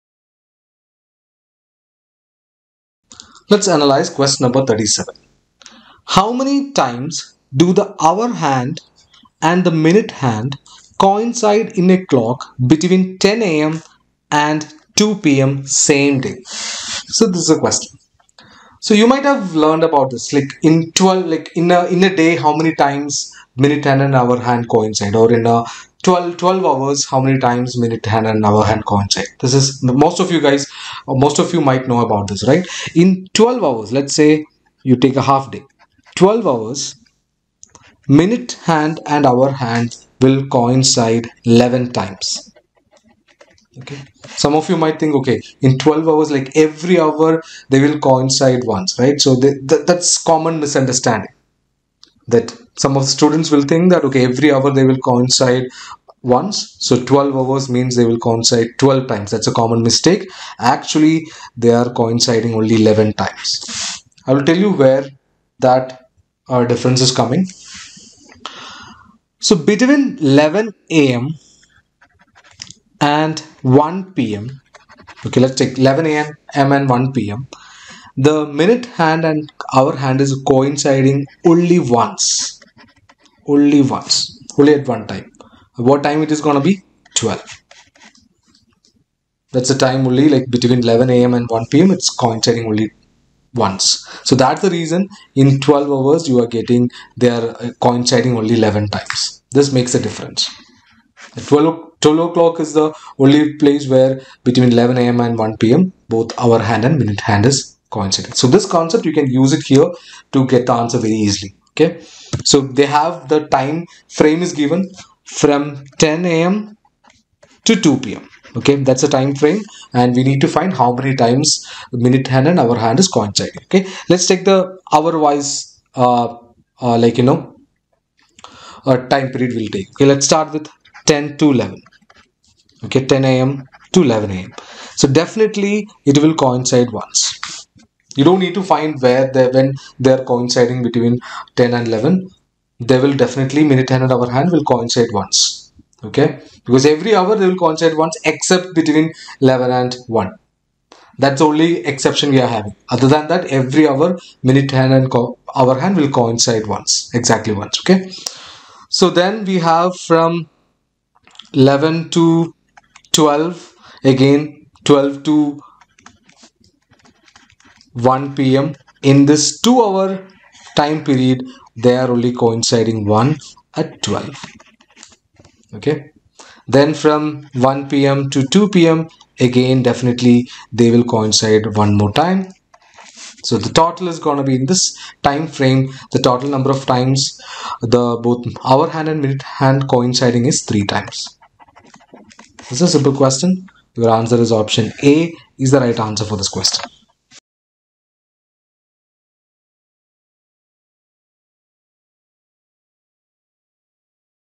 How many times do the hour hand and the minute hand coincide in clock | CSAT 2024 | UPSC | ClearIAS
How many times do the hour hand and the minute hand coincide in a clock between 10:00 a.m. and 2:00 p.m. (same day)?

Let's look at the solution for this problem which was asked in the UPSC CSE Prelims CSAT paper of 2024

Join the ClearIAS CSAT Program to get instant access to already uploaded classes and automatic access to upcoming classes for just Rs.4999.